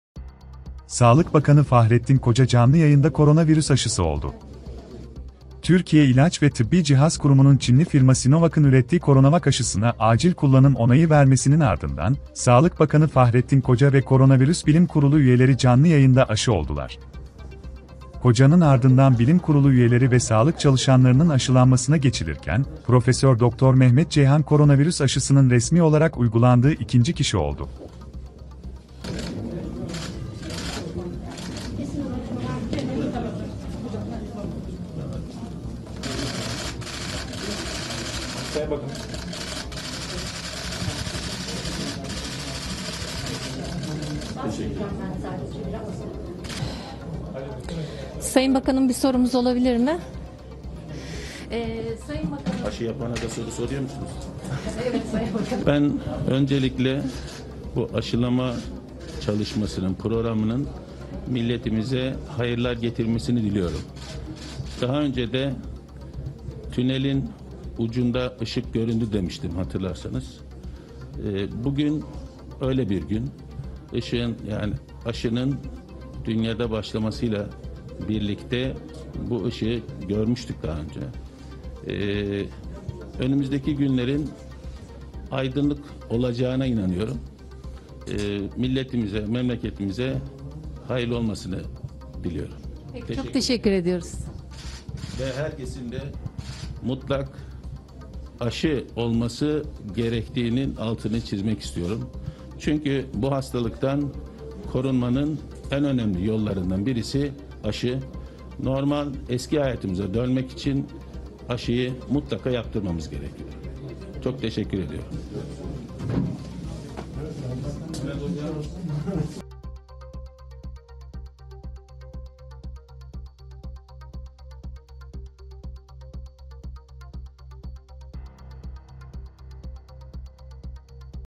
Sağlık Bakanı Fahrettin Koca, canlı yayında Koronavirüs aşısı oldu
Türkiye İlaç ve Tıbbi Cihaz Kurumu'nun Çinli firma Sinovac'ın ürettiği CoronaVac aşısına acil kullanım onayı vermesinin ardından, Sağlık Bakanı Fahrettin Koca ve Koronavirüs Bilim Kurulu üyeleri canlı yayında aşı oldular.